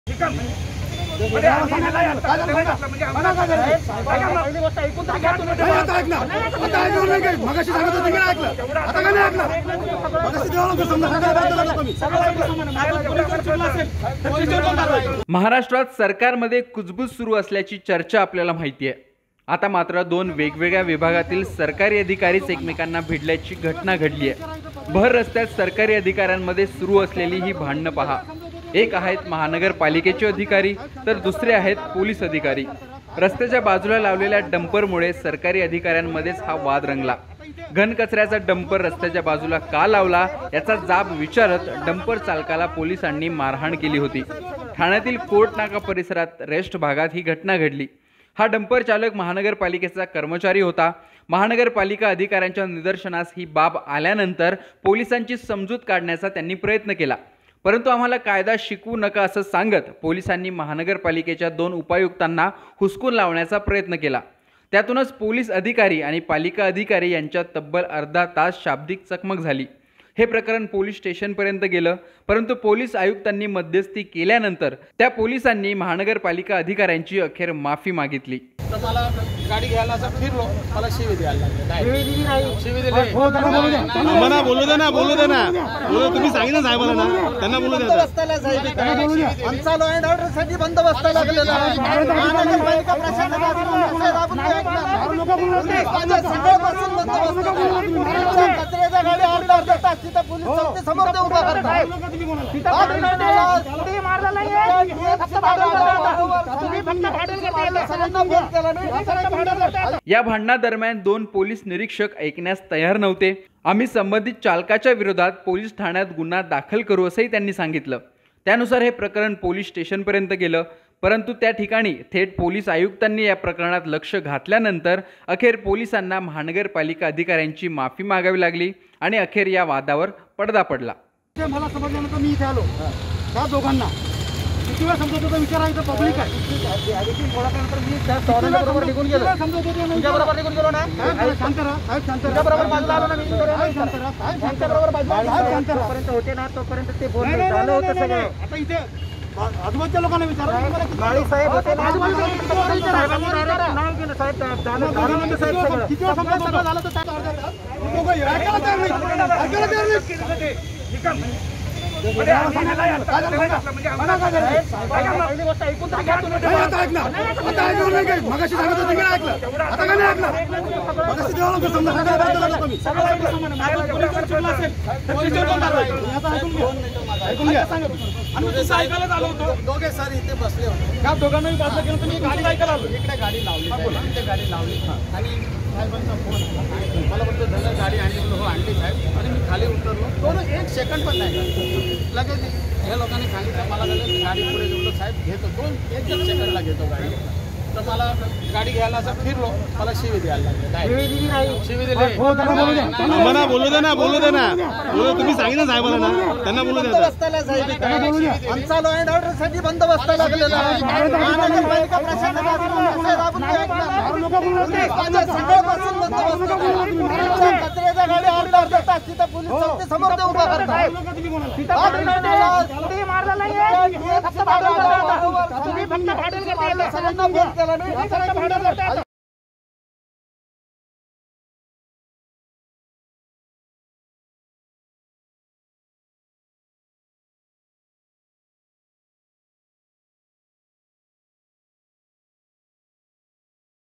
अखेर 'त्या' दोन पोलिसांनी माफी मागितली | Thane Police | Maharashtra News
महाराष्ट्रात सरकारमध्ये कुजबूज सुरु असल्याची चर्चा आपल्या माहितेय.. आता दोन वेगवेगळ्या विभागातील सरकारी अधिकारी एकमेकांना भिडल्याची घटला घडलीय.. भर रस्त्यात सरकारी अधिकाऱ्यांमध्ये सुरु असलेली ही भांडणं. एक आहेत महानगरपालिकेचे अधिकारी तर दुसरे आहेत पोलिस कर्मचारी.. रस्त्याच्या बाजूला लावलेल्या डंपरमुळे सरकारी अधिकाऱ्यांमध्येच वाद रंगला. घनकचऱ्याचा डंपर रस्त्याच्या बाजूला का लावला याचा जाब विचारत डंपर चालकाला पोलिसांनी मारहाण केली होती.  ठाण्यातील कोर्टनाका परिसरातील रेस्ट या भागात घडला. हा डंपरचालक पालिकेचा कर्मचारी होता. पालिका अधिकाऱ्यांच्या निदर्शनास ही बाब आल्यानंतर पोलिसांची समजूत काढण्याचा प्रयत्न केला. परंतु कायदा आम्हाला नका शिकवू असे सांगत, पोलिसांनी पालिकेच्या त्या दोन उपायुक्तांना देखील हुसकावून लावण्याचा प्रयत्न केला. त्यातूनच पोलीस आणि पालिका अधिकारी यांच्यात तब्बल अर्धा तास शाब्दिक चममक झाली. हे प्रकरण पोलीस स्टेशनपर्यंत गेले. परंतु पोलीस आयुक्तांनी मध्यस्ती केल्यानंतर त्या पोलिसांनी पालिका अधिकाऱ्यांची अखेर माफी मागावी लागली.